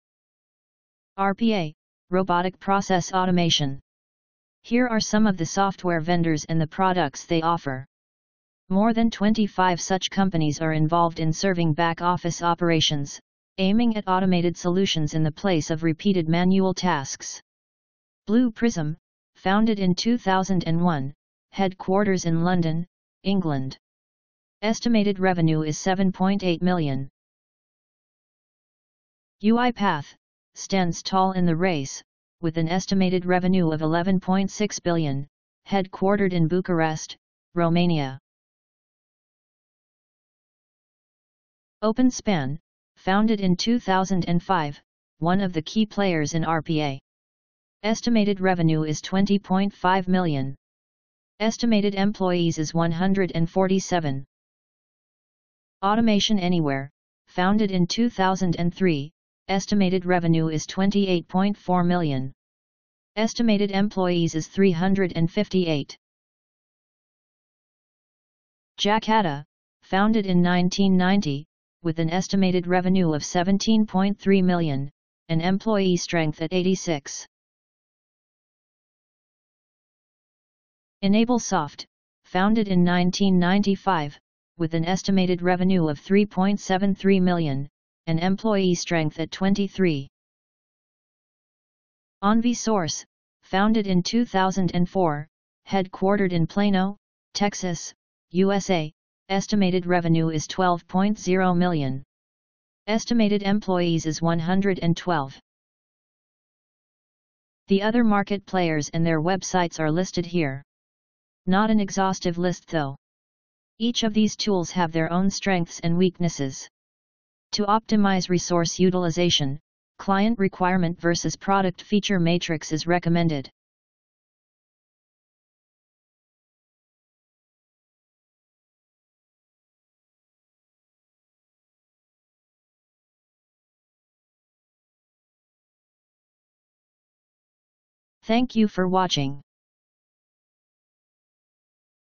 02 RPA Products and Vendors
In continuation with "What is RPA" video, this is produced to introduce various vendors and their comparative strengths. Popular of these vendors and their software available in market.